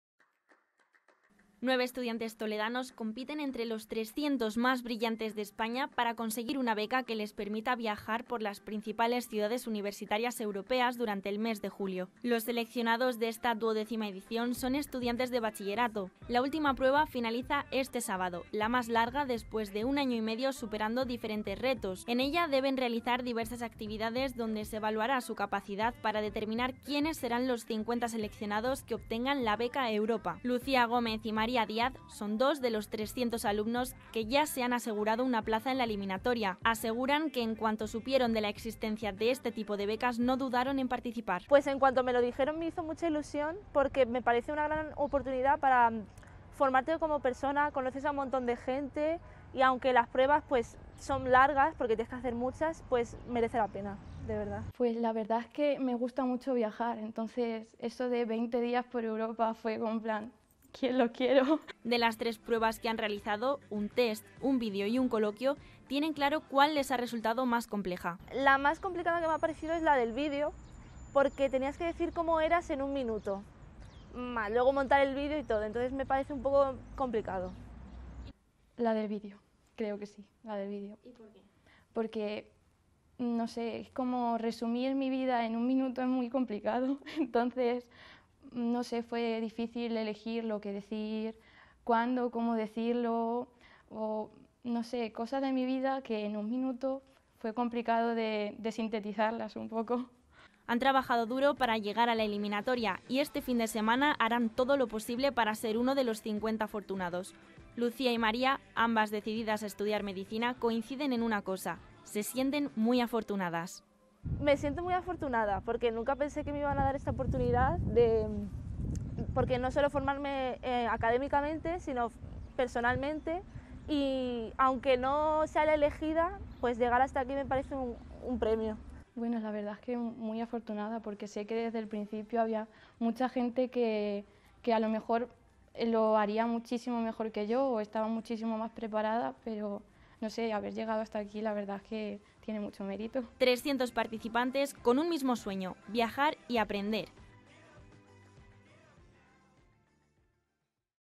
NUEVE TOLEDANOS EN LA FINAL DE LAS BECAS EUROPA
300 estudiantes de toda España compiten este fin de semana para obtener una Beca Europa que les permita viajar por las principales ciudades universitarias europeas, de esta manera se adentran en el espíritu universitario y se implican de una manera activa en la sociedad. Los seleccionados son estudiantes de bachillerato con una nota media de 9’7 y nueve de ellos son de Toledo.